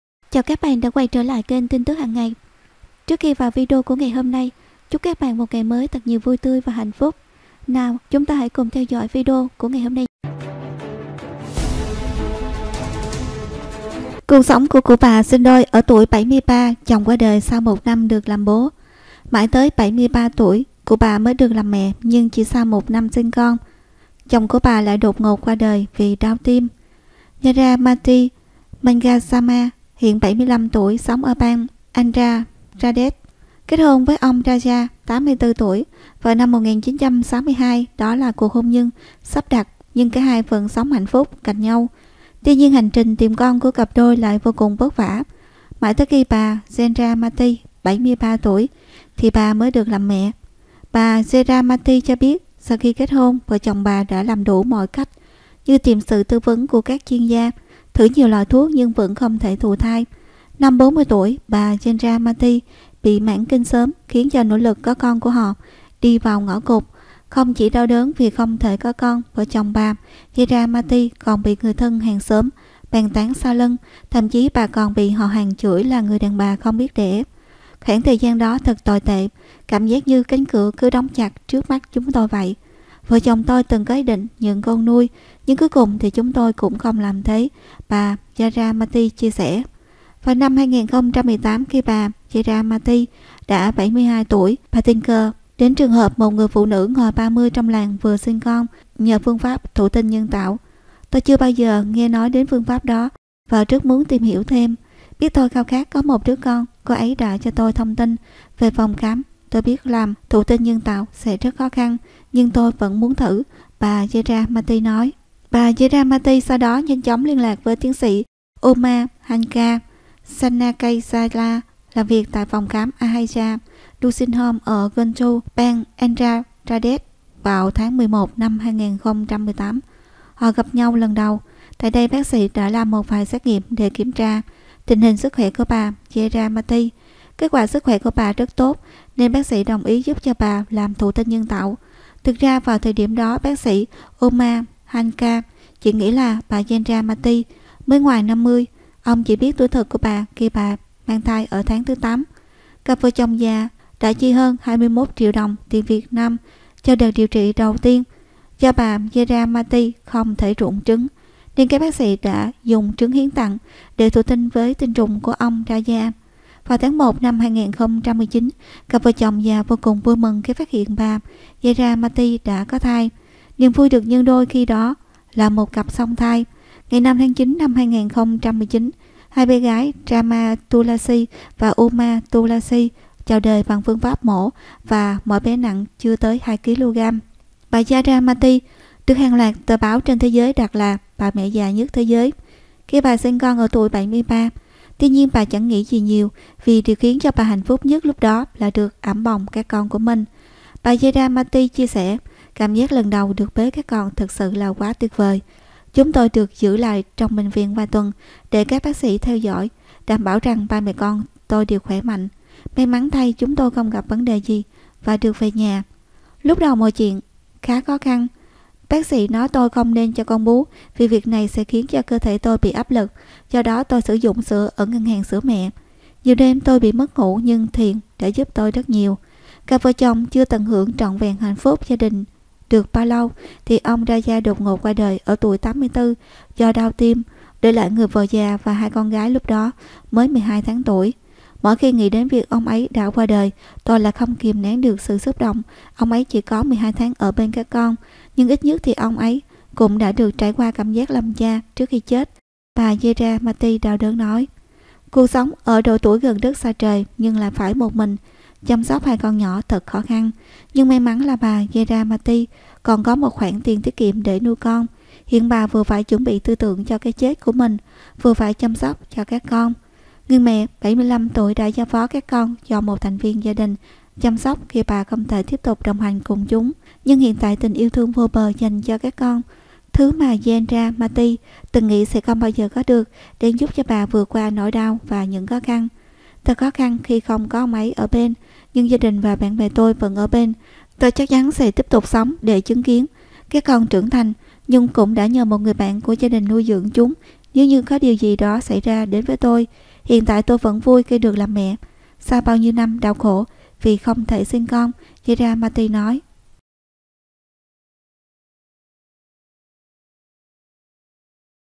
Cuộc sống của cụ bà sinh đôi ở tuổi 73, chồng qia đờl sau 1 năm được làm bố [Tin tức Hằng Ngày]
Cuộc sống của cụ bà sinh đôi ở tuổi 73, chồng qia đờl sau 1 năm được làm bố [Tin tức Hằng Ngày]Tin tức hằng ngày là những thông tin giải trí trong và ngoài nước .